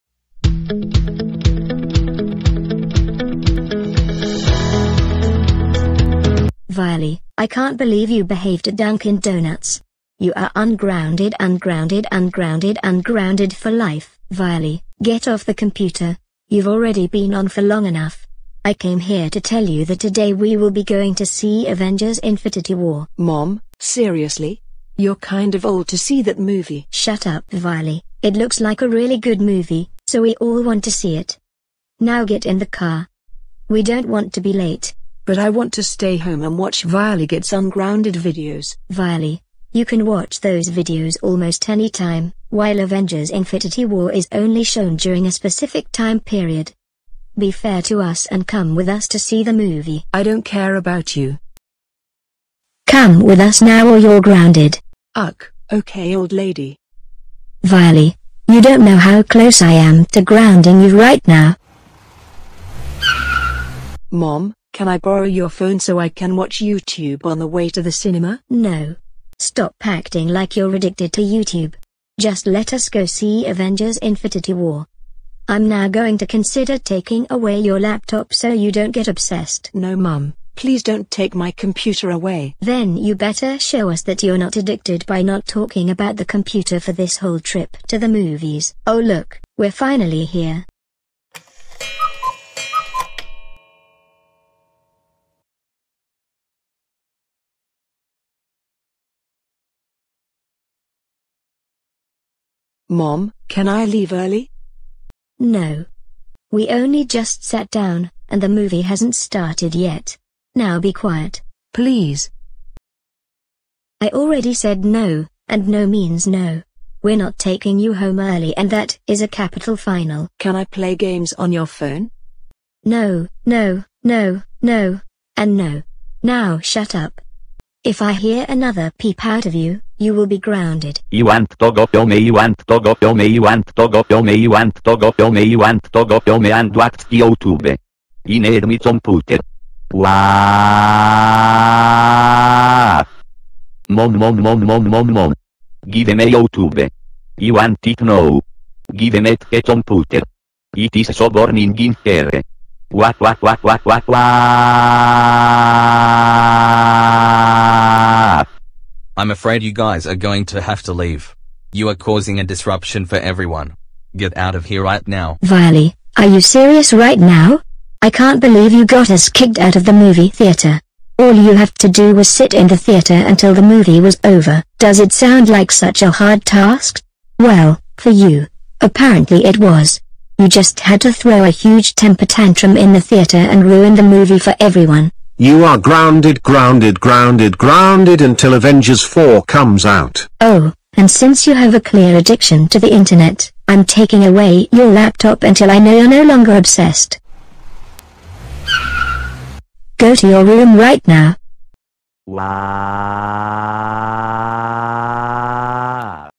Violy Disrupts A:IW And Get's Grounded
Inspired by samster5677
This Is My First Violy Video With The Vyond Logo